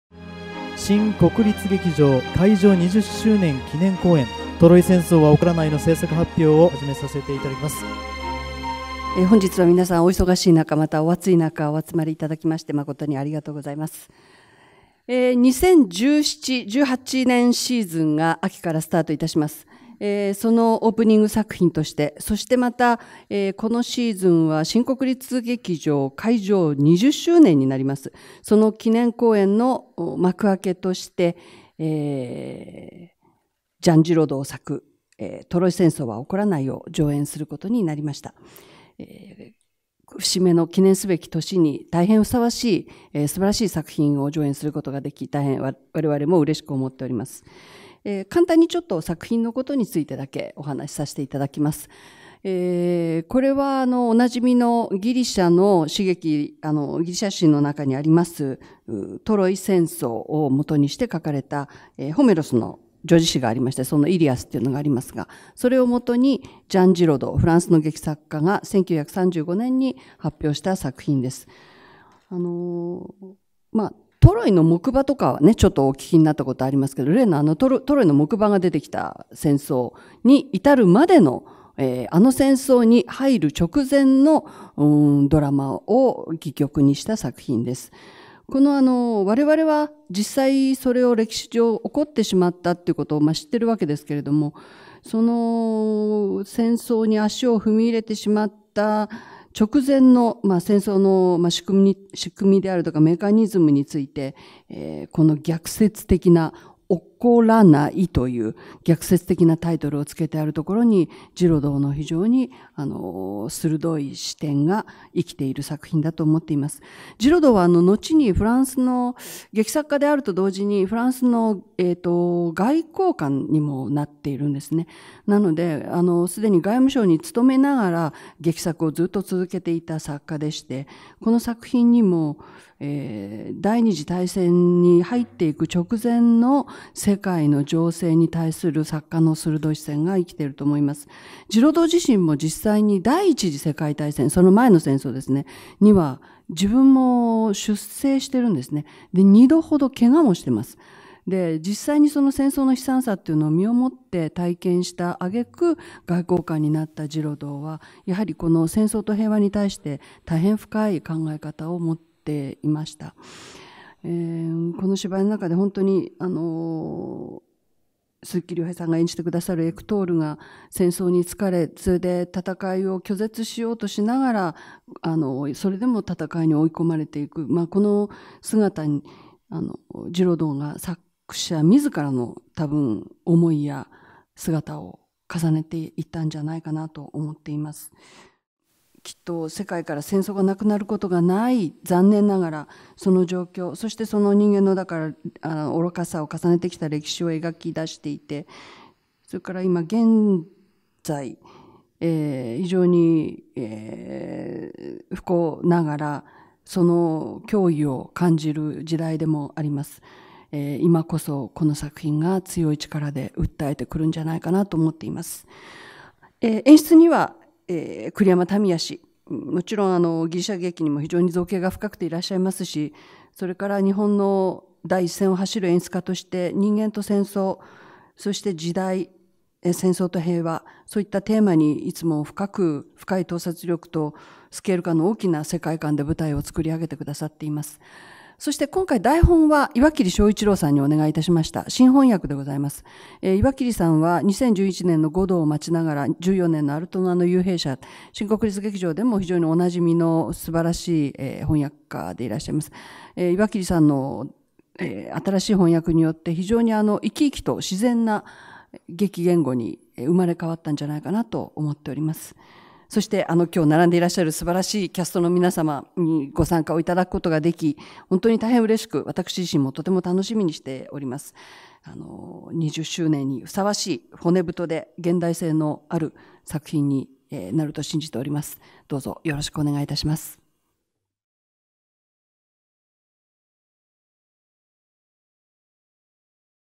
新国立劇場『トロイ戦争は起こらない』制作発表 宮田慶子芸術監督
ジロドゥの不朽の名作。平和を愛する男は戦争を止められるか―

7/29に行われた制作発表より。
「…開場20周年となる記念公演の幕開けとして、素晴らしい作品を上演できることになりうれしく思っております。…戦争が世界からなくなることがないという残念な状況、人間の愚かさを重ねてきた歴史を描き出していて、そして現在不幸ながらその脅威を感じる時代でもあります。今こそ、この作品が強い力をもって訴えてくるんじゃないかなと思います。…」

『トロイ戦争は起こらない』
上演期間／2017.10/5～10/22
会場／新国立劇場 中劇場